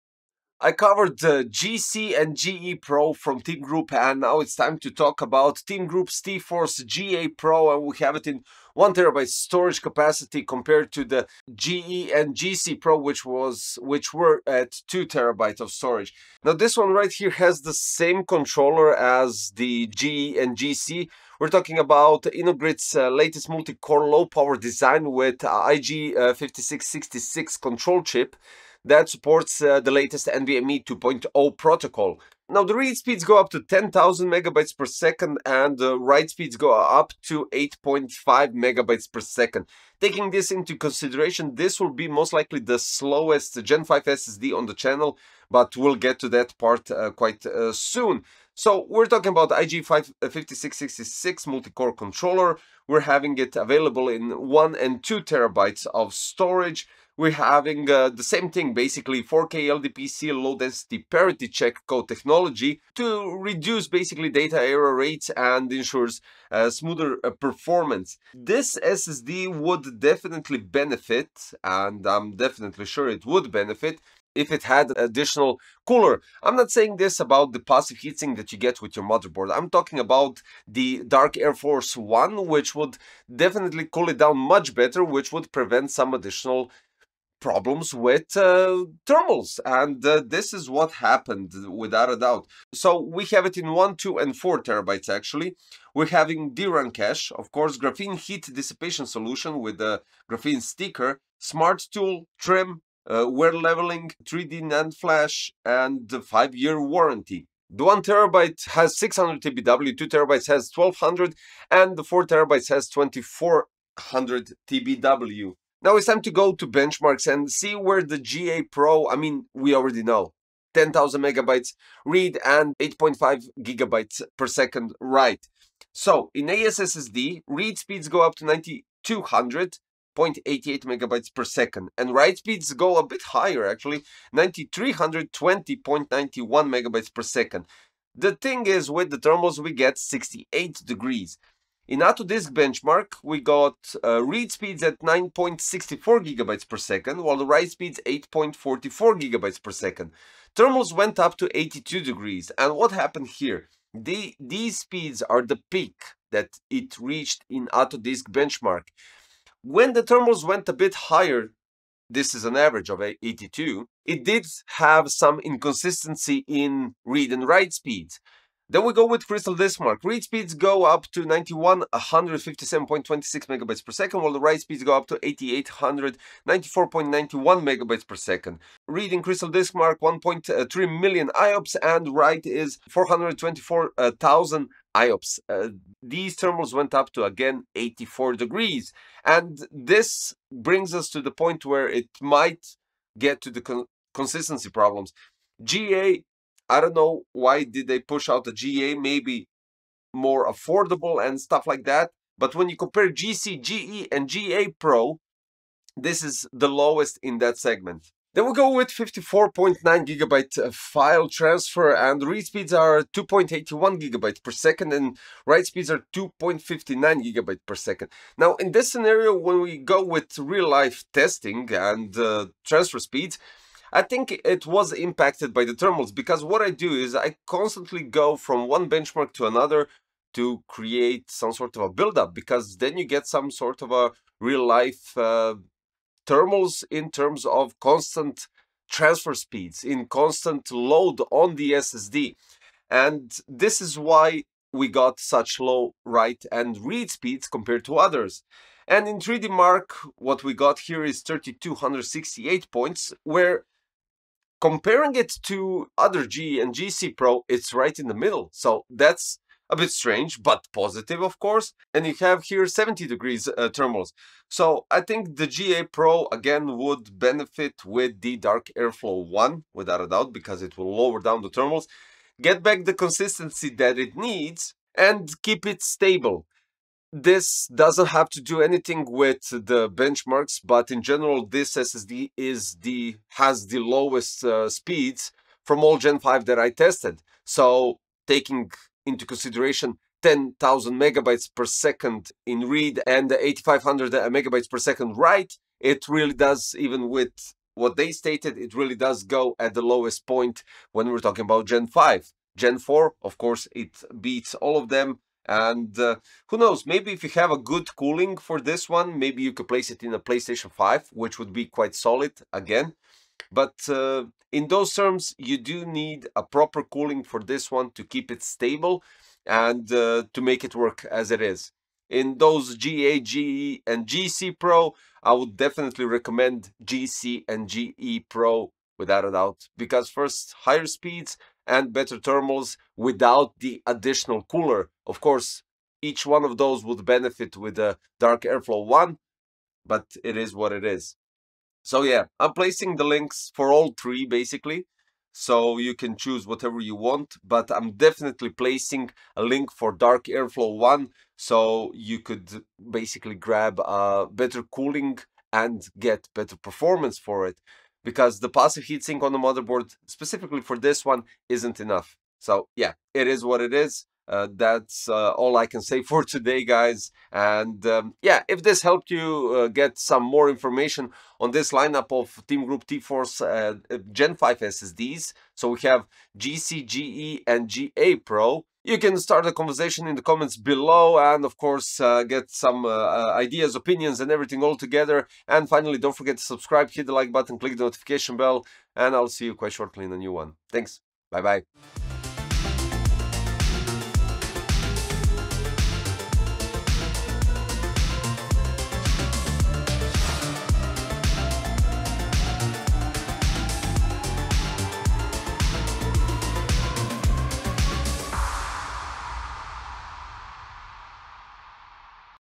TEAMGROUP T-FORCE GA PRO Gen5 1TB - Review
Third Gen5 SSD from the same category, we are having TEAMGROUP T-FORCE GA PRO with 1TB of storage, with read speeds up to 10.000MB/s. Comes with Graphene heatsink sticker as any of the GE PRO and GC PRO models, so check it out.

►TEAMGROUP T-FORCE GA PRO 1TB (Graphene)
►TEAMGROUP T-FORCE GE PRO 2TB (Graphene)
►TEAMGROUP T-FORCE GE PRO 2TB (DARK AirFlow I)

My recording gear:
►Lighting System: NANLITE FS-150
►Softboxes: NANLITE SB-PR-90-Q
►RGB Tubes: NANLITE PavoTube II 6C